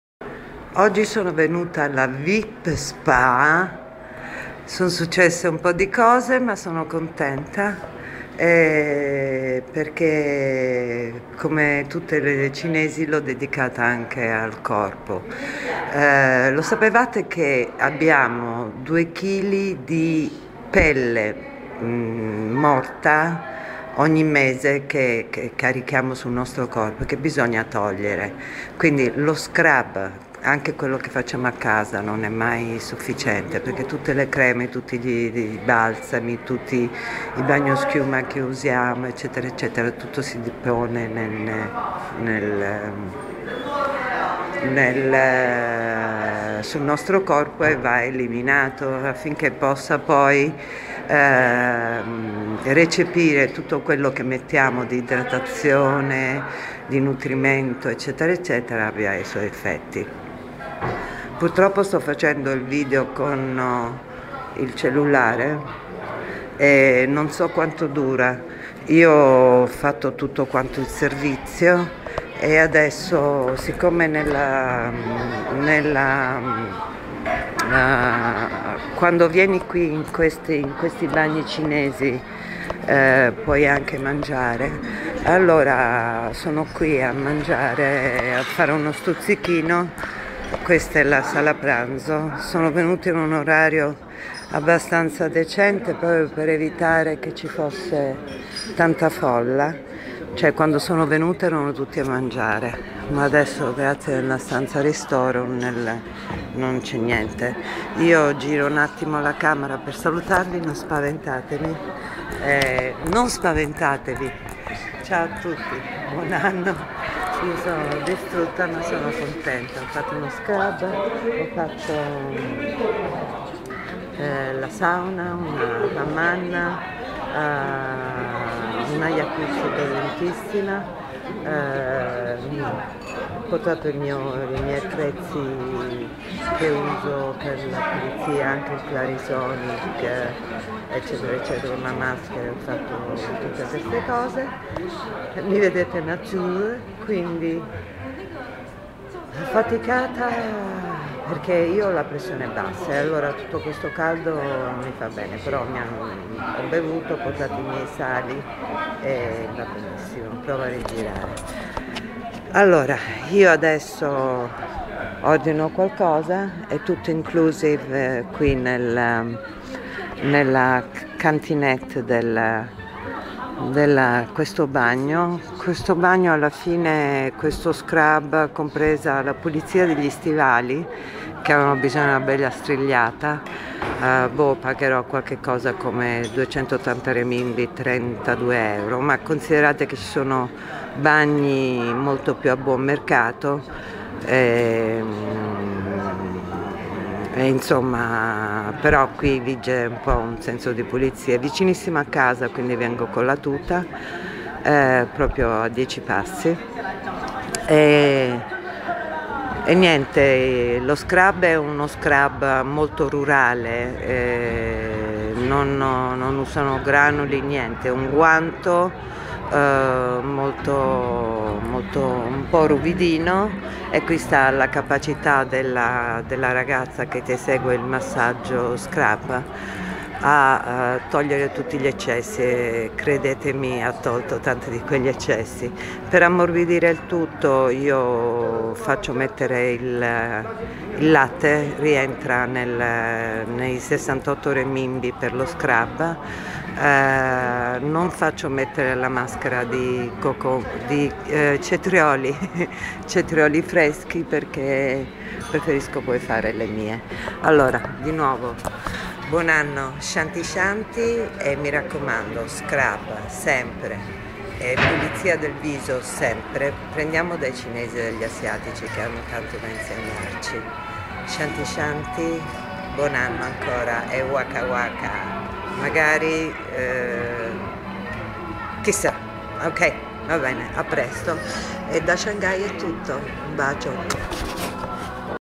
Terzo giorno..."Chinese Bath"
DA LEGGERE PRIMA DELLA VISIONE!
per risollevare lo "spirito" ed il "corpo". la Primavera ci ha portato -3 gradi!!!:-)
tornata a casa ho fatto un po' di ninna! Che meraviglia!
Il nostro corpo ogni mese produce due kg di "cellule morte", che e' necessario eliminare con i cosiddetti scrub. Perche? logico, e' la vera pulizia della pelle, che serve alla pelle stessa far "ricevere" quel ben di Dio che ci spalmiamo, per pulirla, idratarla e nutrirla. Anche gli olii doccia non eliminano le cellule "morte", e quindi bisogna eliminarle con lo scrub. E spesso, quello casalingo non e' sufficiente.
Nei cosiddetti "bagni" cinesi, fai una doccia per entrare nella jacuzzi a 42 gradi, alternando con un passaggio in acqua gelida. Poi entri in una steam sauna, e quando esci sei pronta per lo scrub che viene eseguito con un semplice guanto monouso ed il corpo viene inumidito di volta in volta con del normale latte tiepido. Dopo 40 minuti, svegliandoti da questa goduria, saluti le tue cellule (che schifo) che rimangono sul lettino, fai un veloce doccia e la signorina ti spalma di latte e sei pronta per rientrare nella steam sauna. Dopodiche, esci e finisci da sola il rituale,capelli, viso. indossi il vestitino hawaiano (che io non faccio) e ti rechi al ristorante dove fai uno spuntino. Il mio?: succo di aloe e jaozi in zuppa. Pagato il conto, indossati i mie stivali lucidati e via con la prima sigaretta verso casa.
PS: Qualora, dopo il bath, si volesse proseguire, nei piani superiori ci sono i saloni per massaggi, mani/pedi, facial, waxing, etc etc. ed anche le stanze per trascorrere una serena notte!